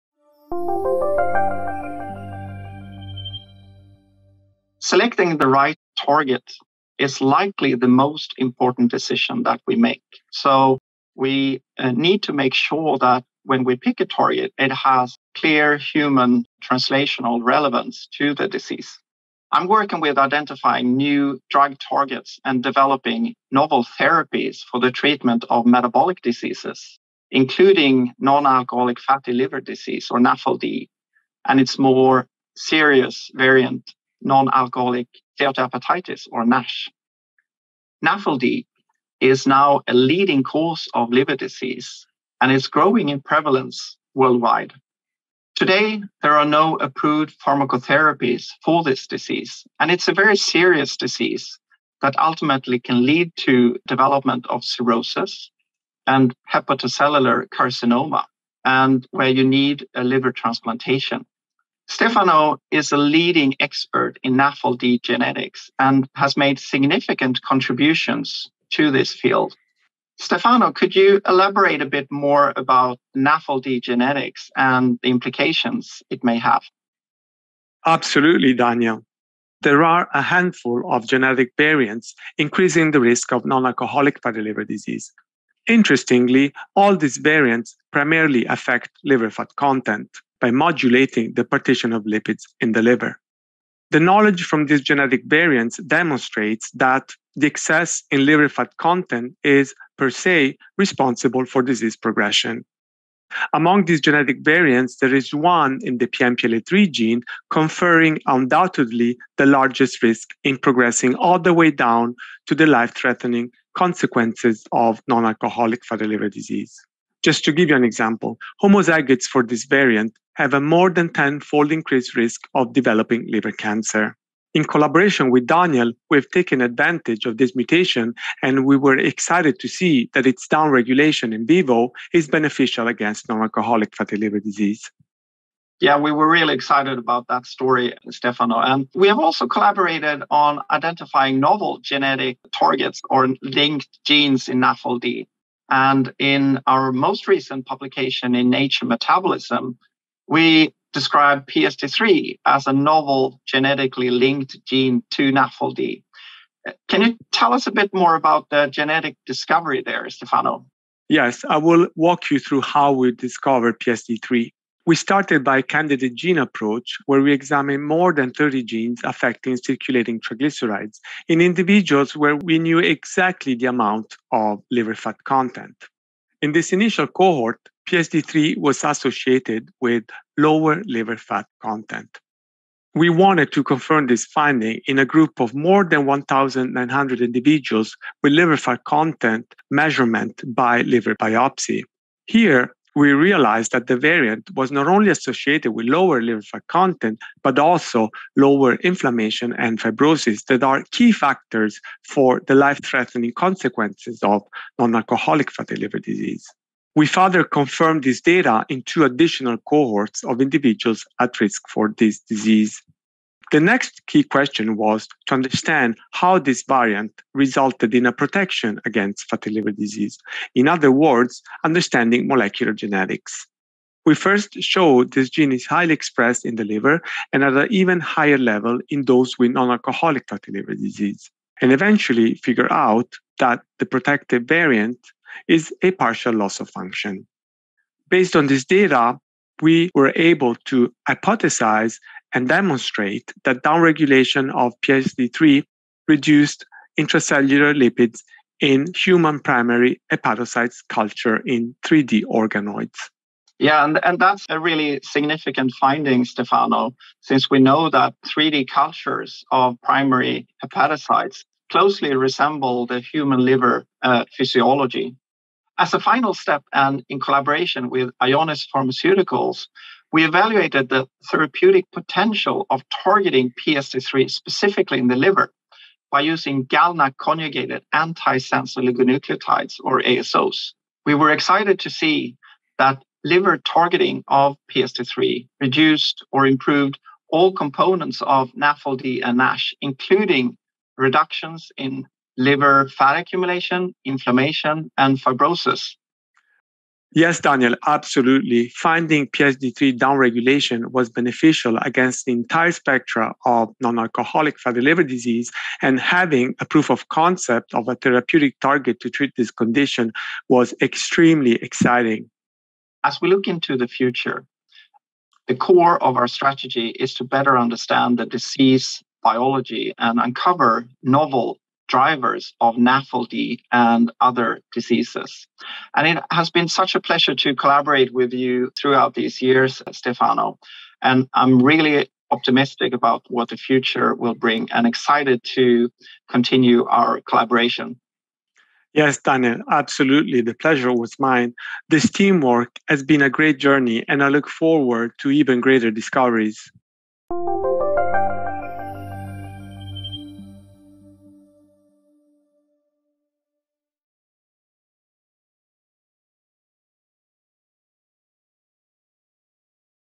Discovering novel genetic targets for fatty liver disease
We have been collaborating with scientists at the University of Gothenburg to identify novel therapeutic targets for non-alcoholic steatohepatitis (NASH), a chronic liver disease which can have serious, life-threatening consequences.

Our recent high impact publication in Nature Metabolism (January 2022, 4, p60-75) reports exciting results from this research with the identification of PSD3, a new genetic target that has been shown to be associated with fatty liver disease. This is an important discovery which may lead to the development of a precision medicine for the future treatment of NASH.

0:38 The NASH Landscape
1:02 Understanding NASH genetics
1:42 PNPLA3 gene
2:25 PSD3 gene
5:14 Looking to the future